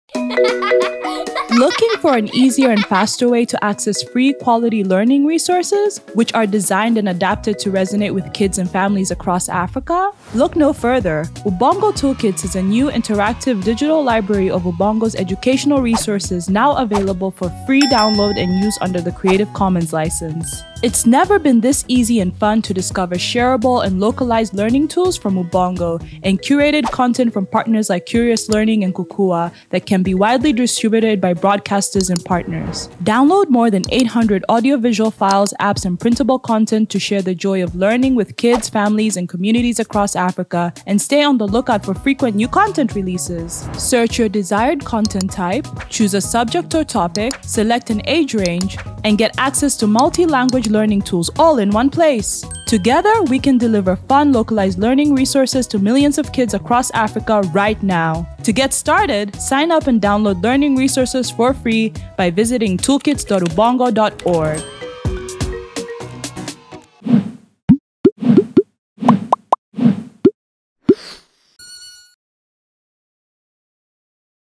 Introducing Ubongo Toolkits | Free Educational Resources for Africa's Learners
Looking for an easier and faster way to access free quality learning resources? Download the Ubongo Toolkits - a new interactive digital library of Ubongo's educational resources. To get started, sign up and download learning resources for free by visiting

Send a message with the word UTU to by whatsapp to learn more about how set up and join a UTU club in your community.